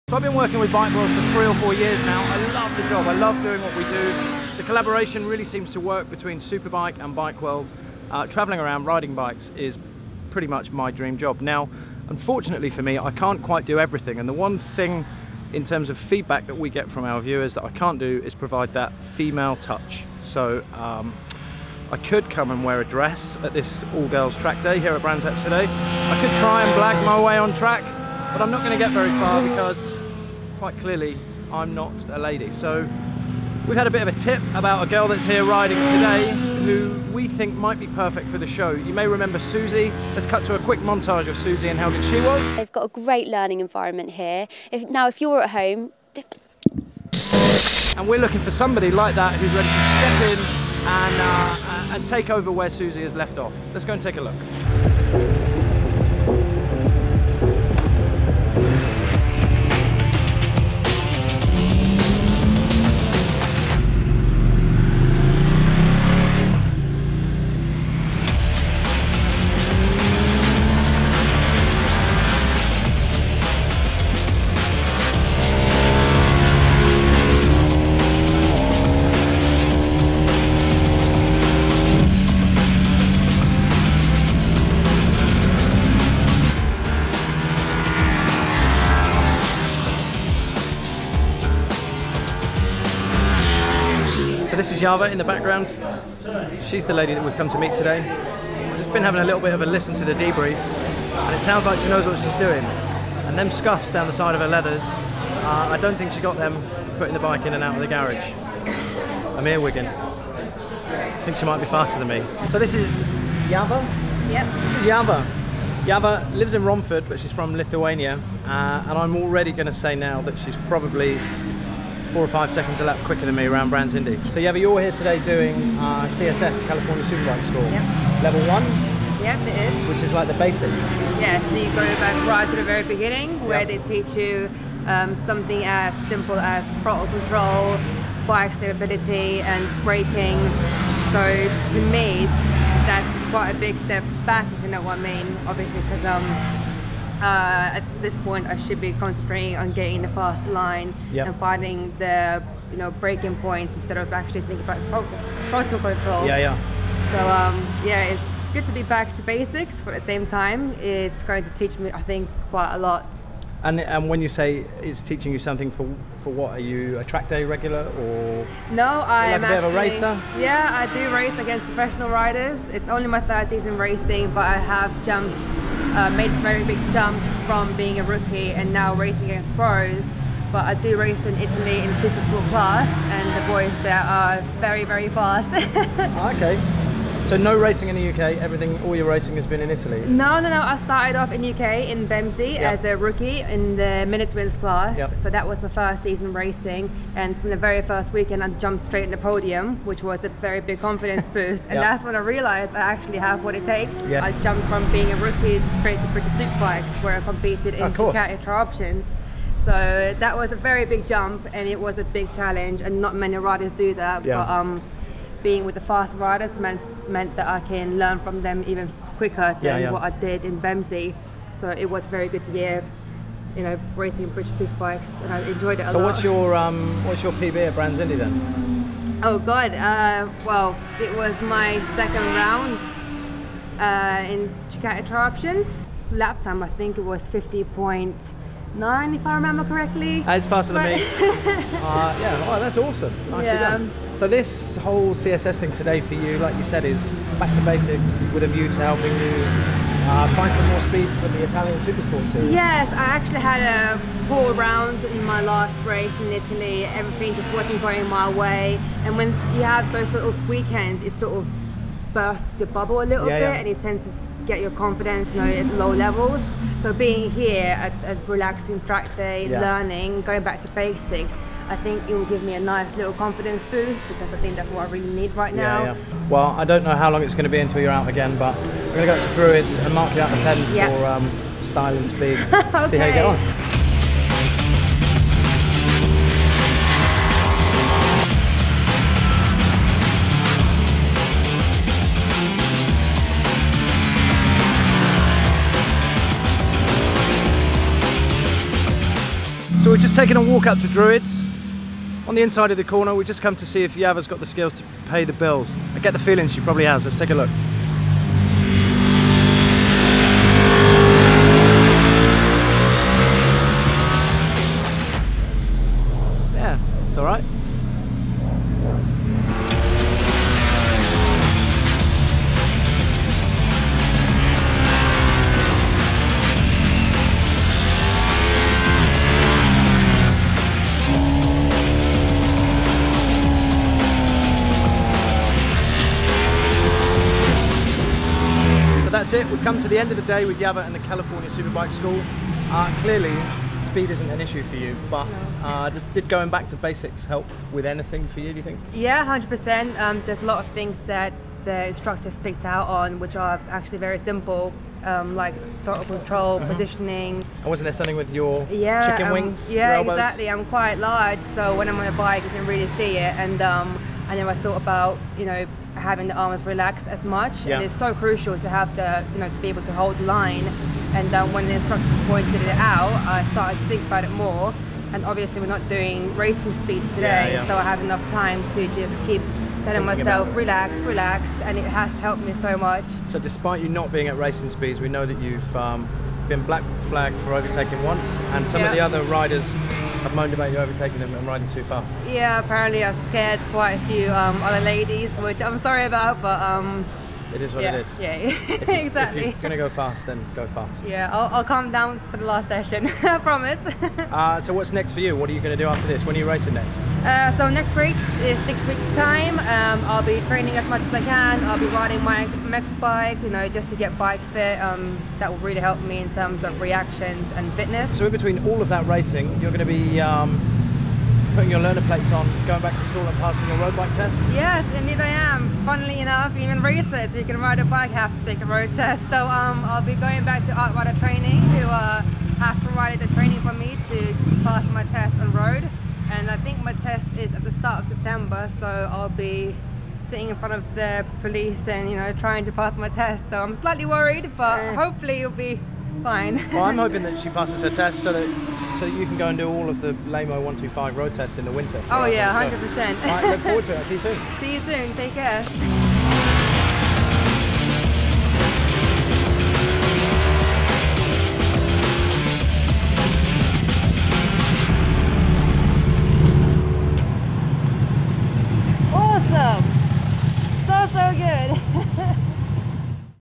We meet Ieva Baublyte At CSS Brands Hatch
Ever since Suzy headed off to the US we've been missing the female touch on here Bike World.  We heard that Ieva Baublyte would be at the California Superbike School at Brands Hatch, so we headed down to meet her.